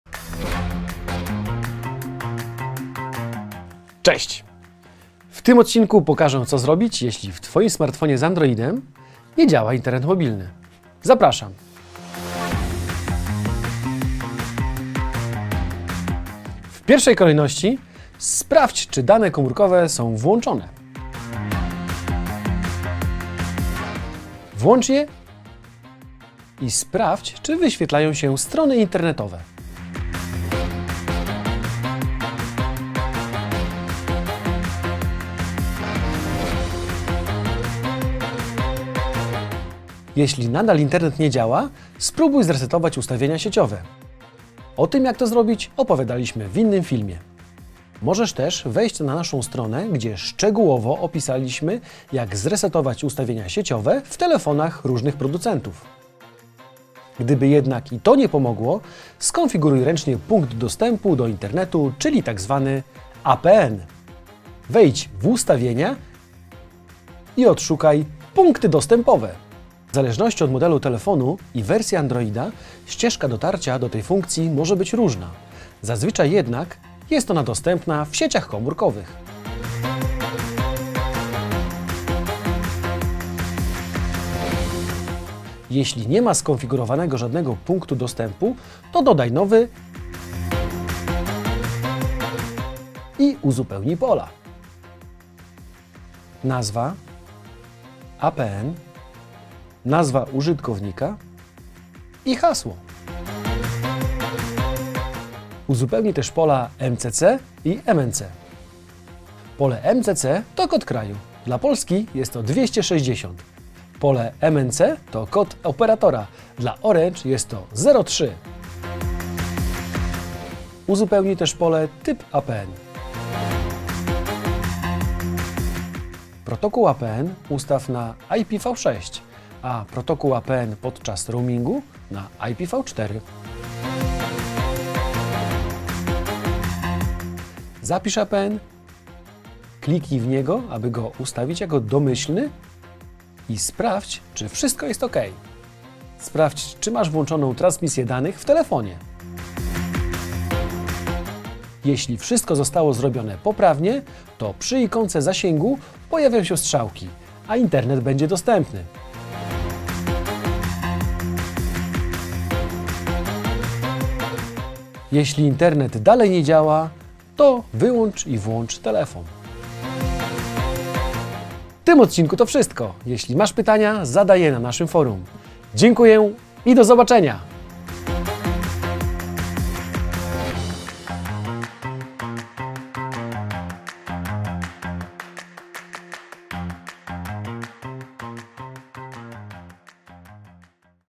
💡 ORANGE EKSPERT - 👉 Jak skonfigurować internet mobilny w telefonie? 📱
Nie działa Ci internet w smartfonie z Androidem? Zobacz co należy sprawdzić i skonfigurować.

00:00 Wprowadzenie
00:16 Krok 1 - włącz dane mobilne
00:40 Krok 2 - zresetuj ustawienia sieciowe
00:57 Krok 3 - skonfiguruj APN
02:52 Zakończenie